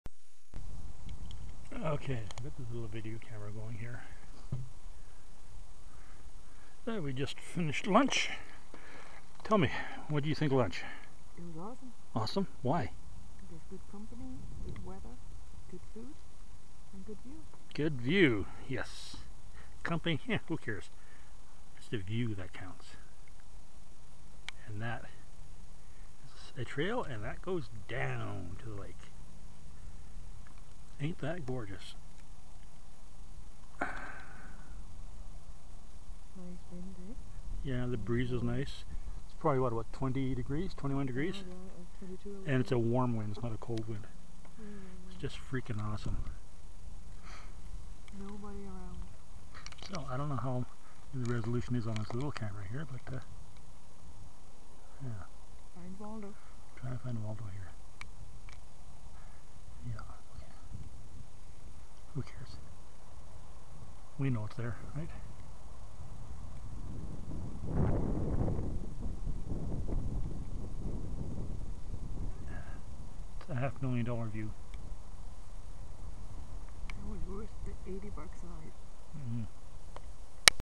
0006 Picnic at the Bathtub
Kelly's Bathtub is a small "Kettle Lake", part of Jarvis Lake, and one hell of a viewpoint for a picnic. Unfortunately, the sun was directly in front of our view, so a lot of colours are washed out. As well, these last 6 videos were taken with a small pocket video recorder with low resolution - probably barely 720, if that.